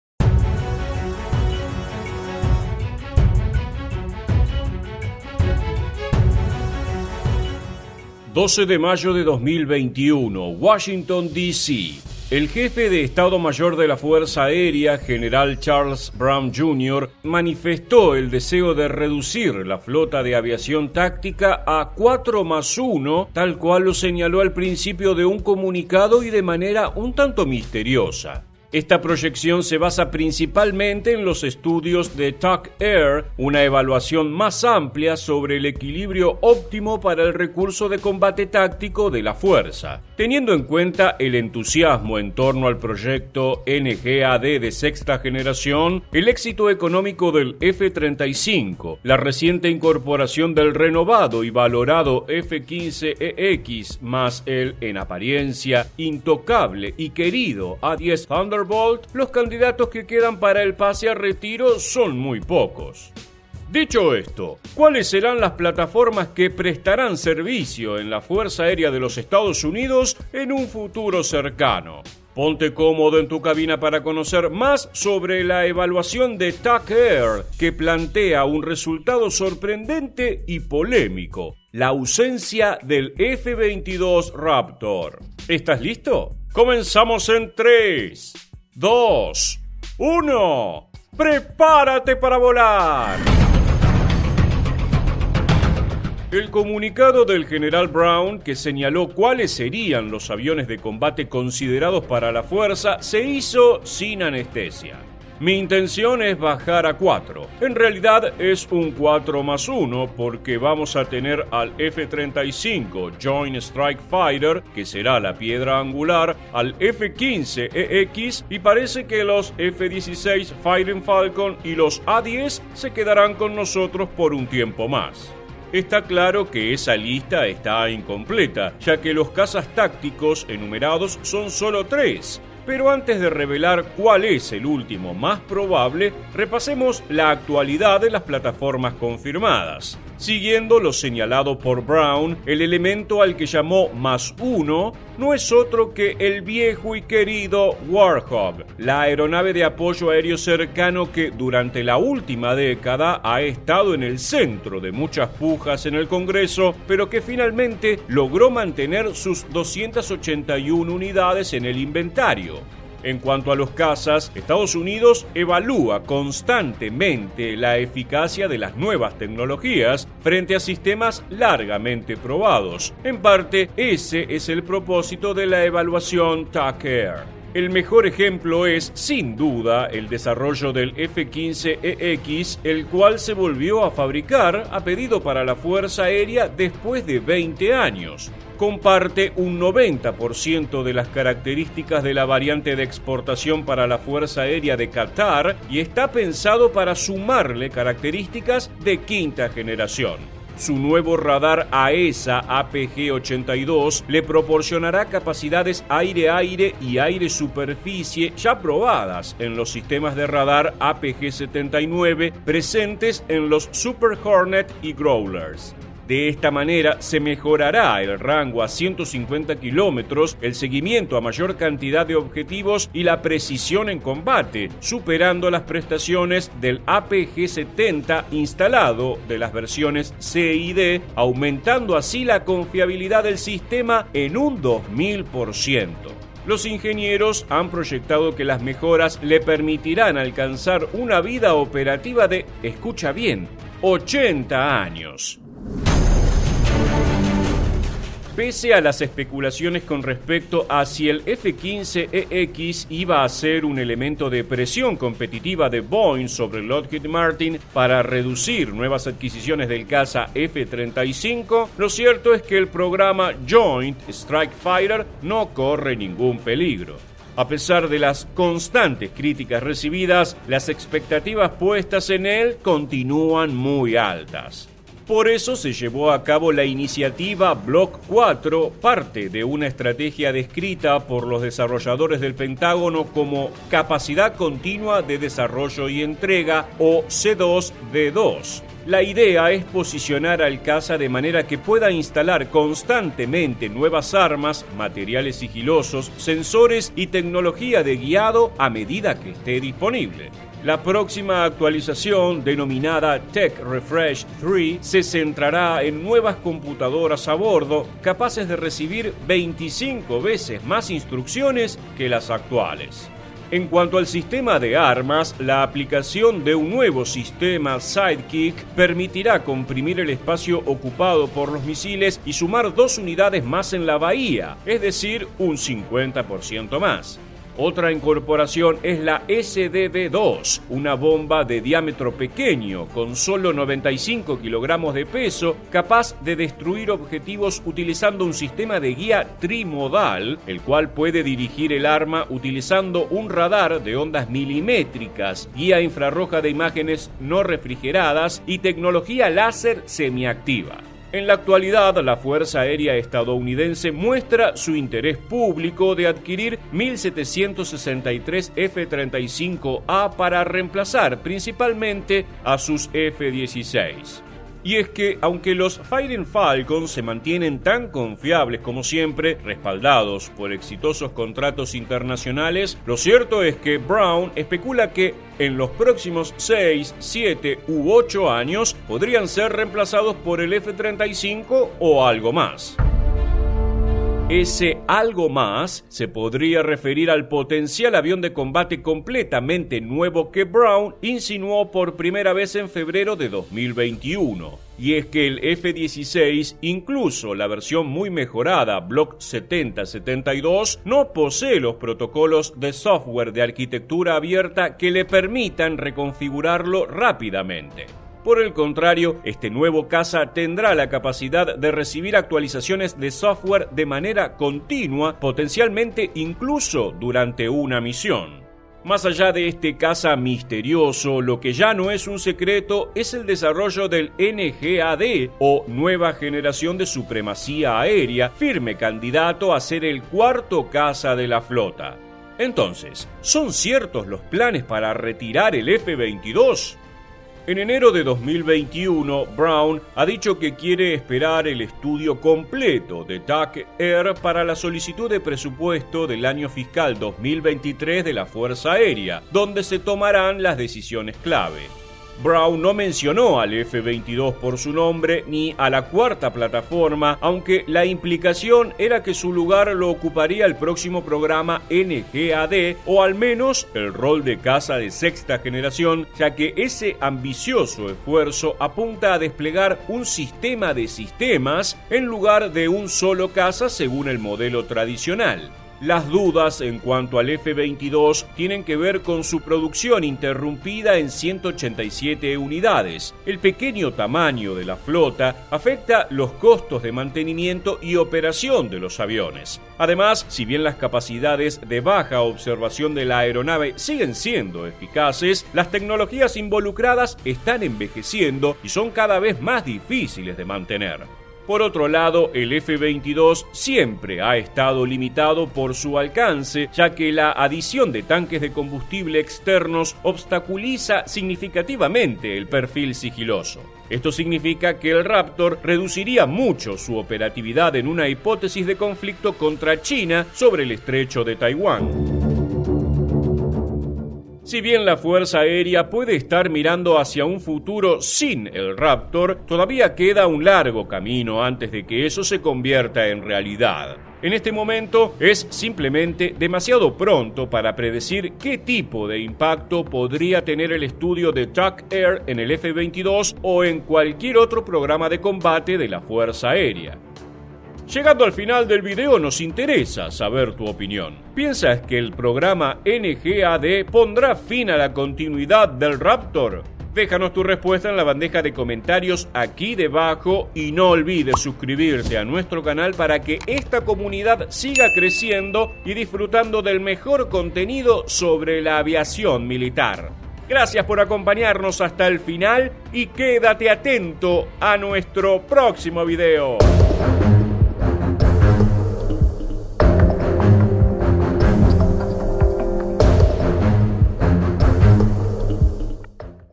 ¿Por qué EE.UU descartará al F-22 Raptor? | Evaluación TacAir
¿Sabes por qué EE.UU descartará al F-22 Raptor? ¡Si te interesa saber más de la Evaluación TacAir, no te pierdas este video!

¡También estamos en Facebook con contenido exclusivo!